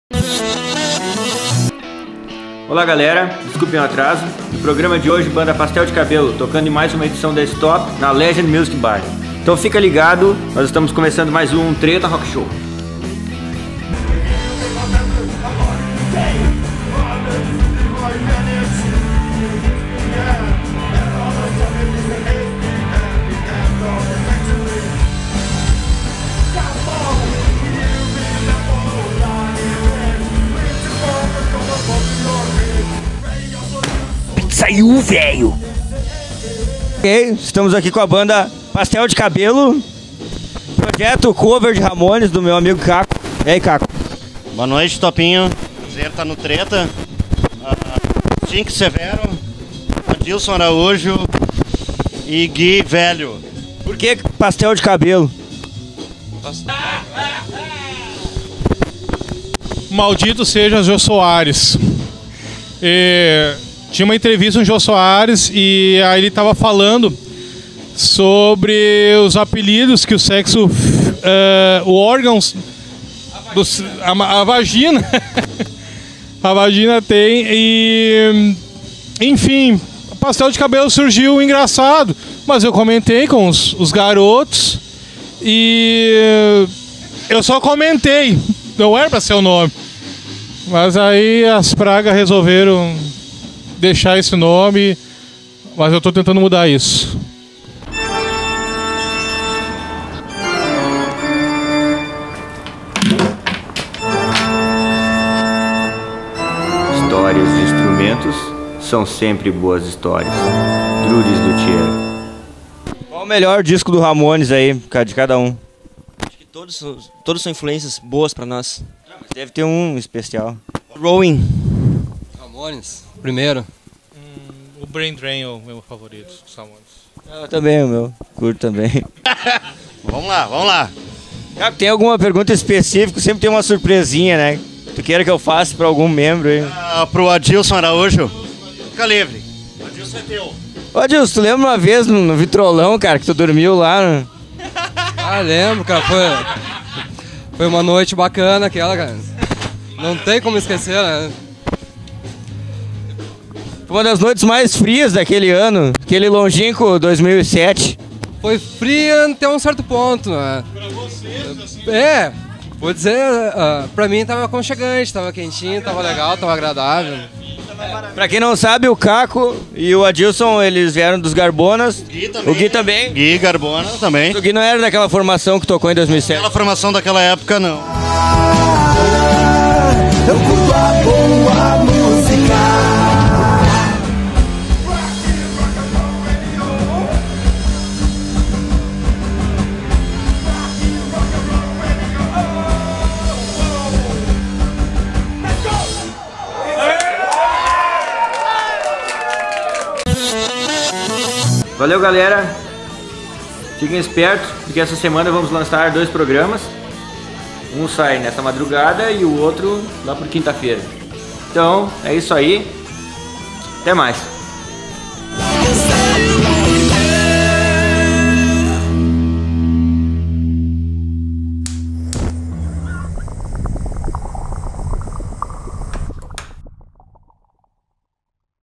#47 TRETA ROCK SHOW
Entrevista com Pastel de Cabelo